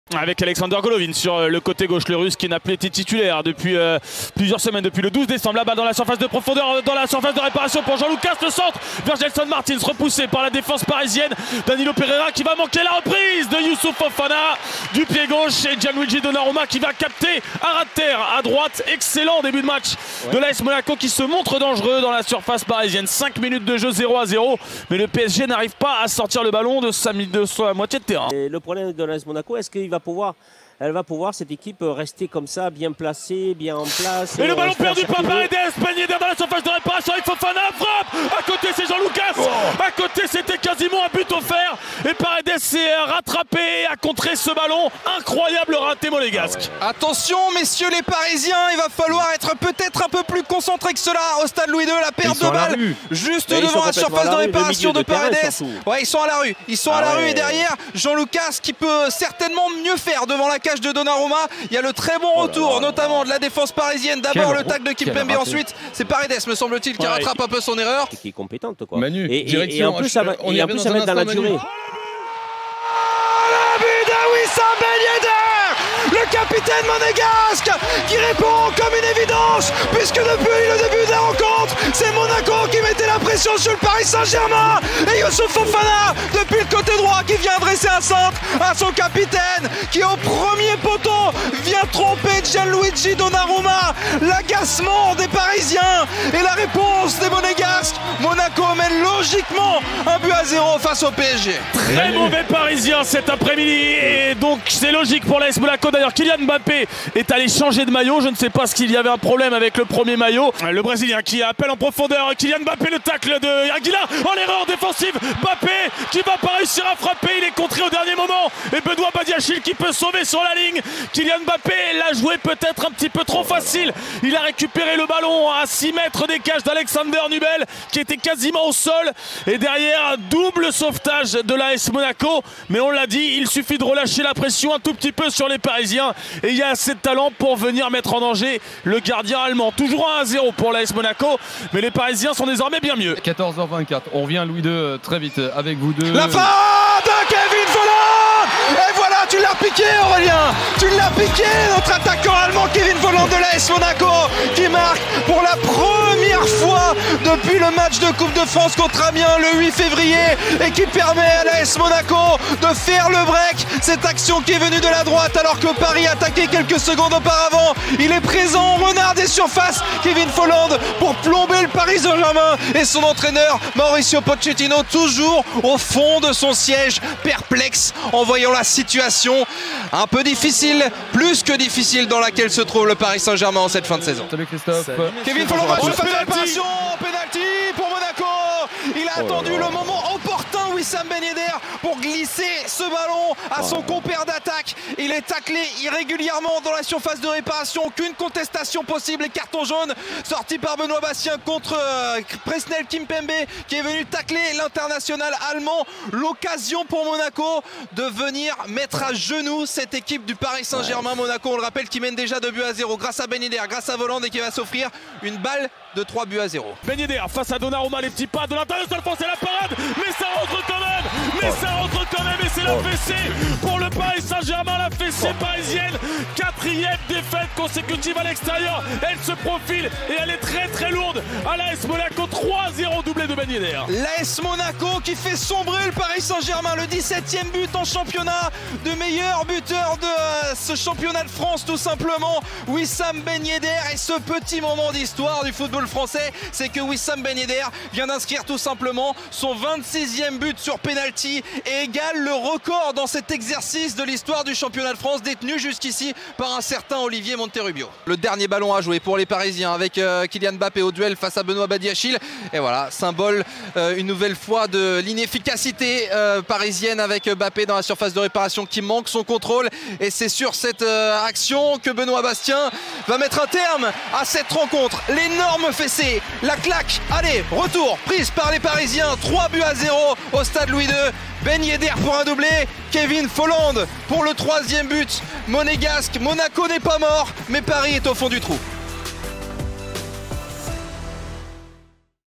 Monaco 3-0 PSG : Paris sombre, 3e défaite de rang à l’extérieur en L1 ! (avec les commentaire RMC)
Paris ne sait plus gagner loin du Parc des princes. Pire, dimanche le club de la capitale s’est lourdement incliné à Monaco (3-0, 29e journée). Une troisième défaite de suite à l’extérieur en Ligue 1 (après Nantes et Nice) qui fait tâche. Revivez les temps forts du match avec les commentaires RMC.